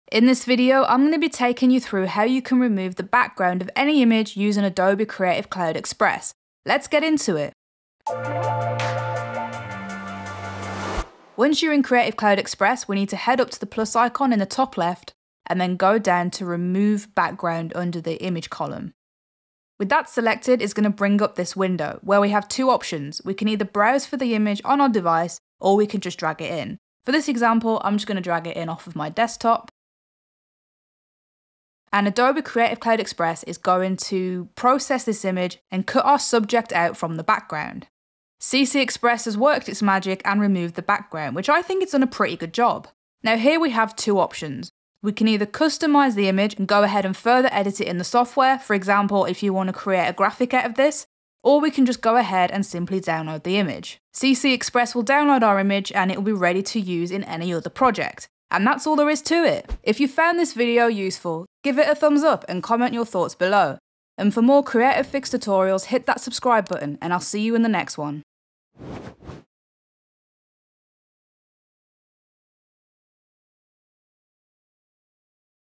How To Remove The Background From An Image In Adobe Express
Learn how to remove the background from any image using Adobe Creative Cloud Express in just a couple of clicks!

Timestamps ▶
00:00 - Intro
00:13 - How to find background removal tool in Adobe CC Express
00:27 - How to import your image for background removal
00:46 - How to download your new image